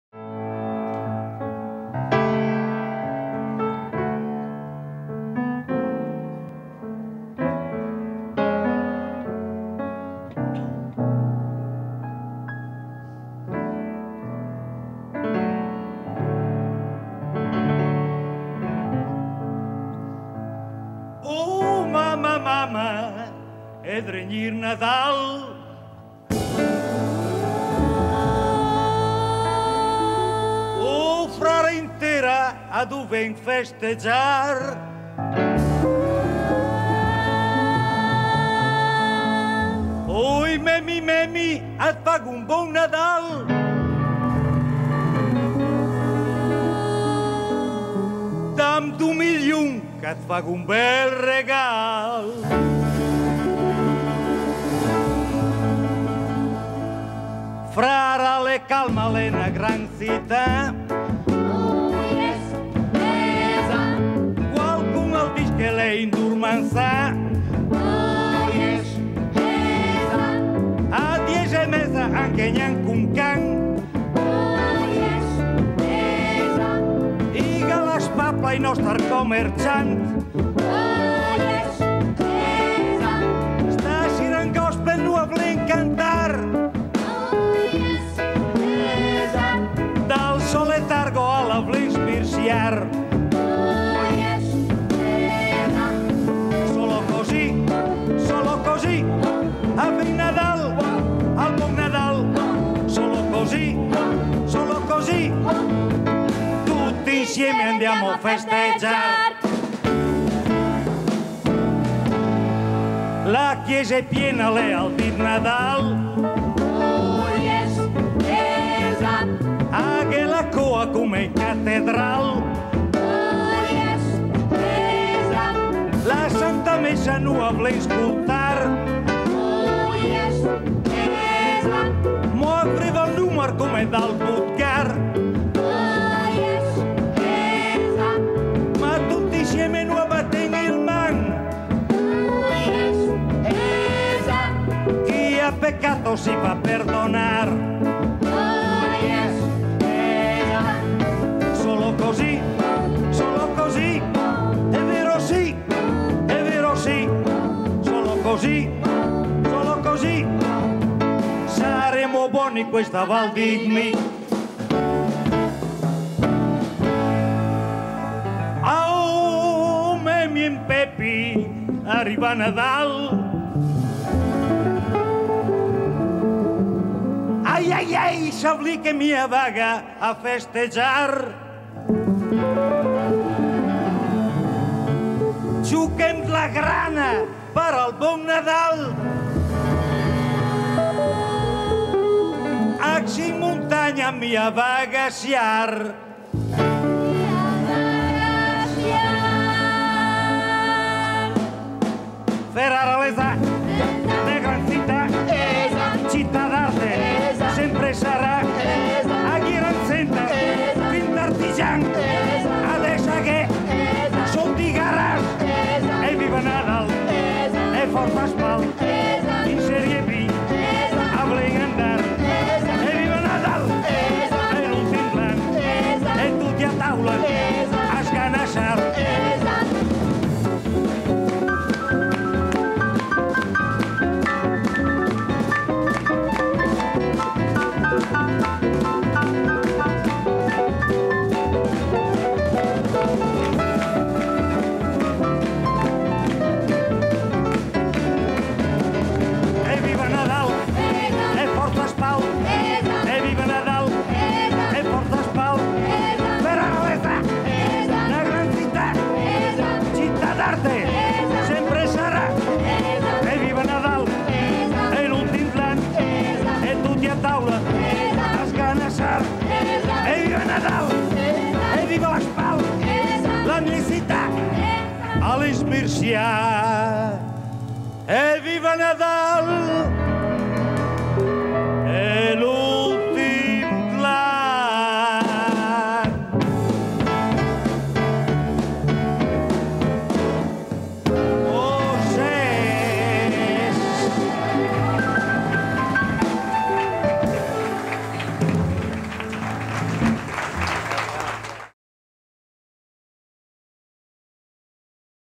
Alfio Finetti in versione Gospel music dialetto Ferrarese
Telestense anni '90

Alfio Finetti in versione Gospel music dialetto  Ferrarese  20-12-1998
accompagnati dal Santa Claus special Quartet composto da:
 Ares Tavolazzi, Ellade Bandini, Antonio Cavicchi, Massimo Mantovani.